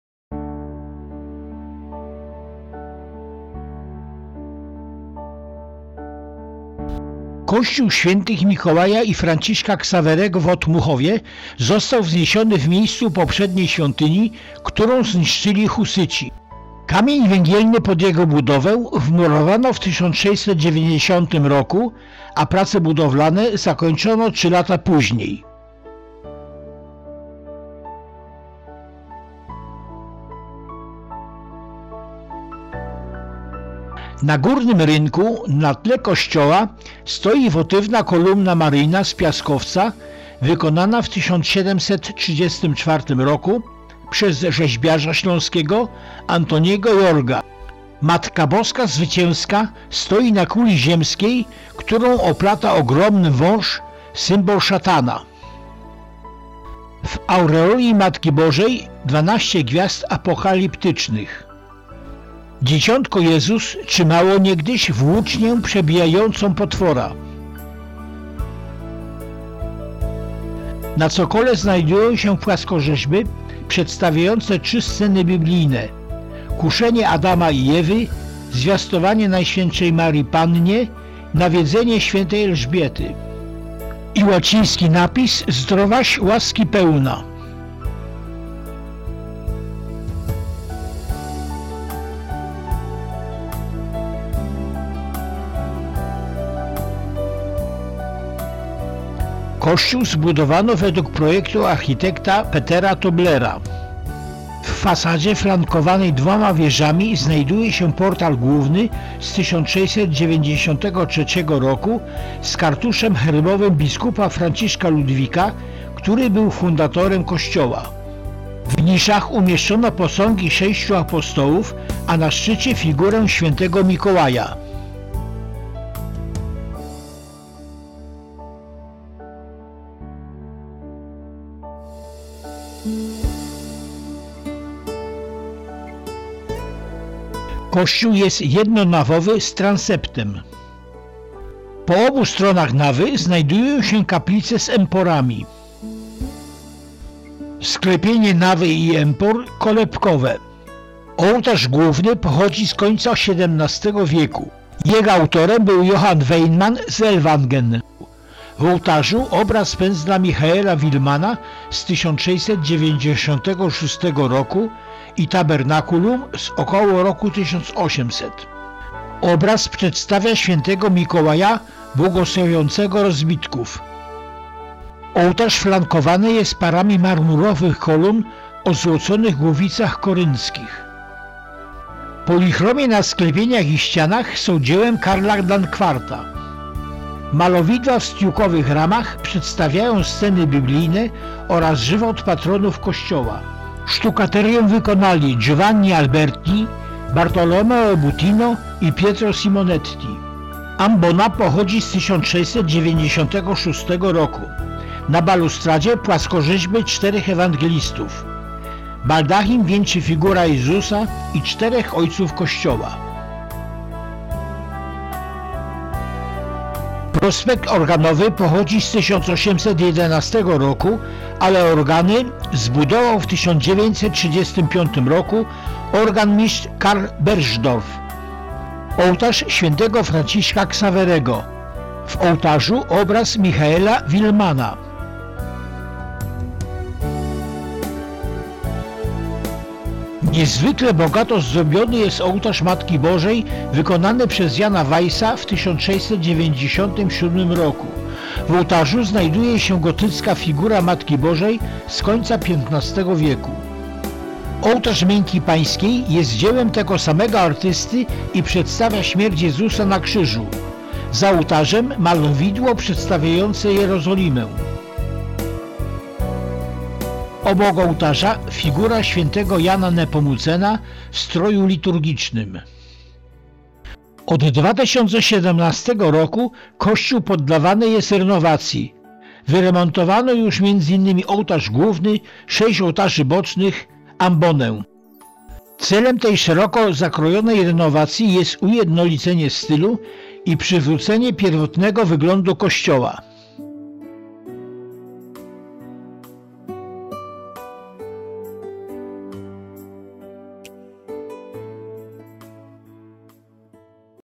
Kościół św. Mikołaja i św. Franciszka Ksawerego w Otmuchowie.
Wideo z napisami (Video with subtitles)
Kościół świętego Mikołaja i świętego Franciszka Ksawerego to rzymskokatolicki kościół parafialny należący do dekanatu Otmuchów diecezji opolskiej.
Pierwsza świątynia w tym miejscu była wzmiankowana w 1235, zniszczona podczas wojen husyckich została odbudowana w 1466. Kamień węgielny pod budowę obecnej świątyni został wmurowany w 1690 roku, natomiast prace budowlane zakończyły się w 1693 roku. Architektem kościoła był Johann Peter Tobler. Polichromię wykonał Karol Dankwart (znany m.in. z prac w bazylice św. św. Jakuba Starszego i Agnieszki w Nysie), przedstawiają sceny z życia obydwu patronów, Matki Bożej i świętych. Ołtarz główny został wykonany przez Johanna Weinmanna z Ellwangen i poświęcony w 1701 roku. Sztukaterie to dzieło G. Albertiego, B. Mutino i P. Simonettiego.

The Church of Saint Nicholas and Saint Francis Ksawery is a Roman Catholic parish church belonging to the Otmuchów deanery of the Opole diocese.
The first temple in this place was mentioned in 1235, destroyed during the Hussite wars, it was rebuilt in 1466. The foundation stone for the construction of the present temple was laid in 1690, and the construction work ended in 1693. The architect of the church was Johann Peter Tobler. The polychrome was made by Karol Dankwart (known, among others, from his works in the Basilica of St. James the Elder and Agnes in Nysa), depicting scenes from the life of both patrons, the Mother of God and the saints. The main altar was made by Johann Weinmann from Ellwangen and consecrated in 1701. The stucco is the work of G. Alberti, B. Mutino and P. Simonetti.

Muzyka: Michael Dlugosh.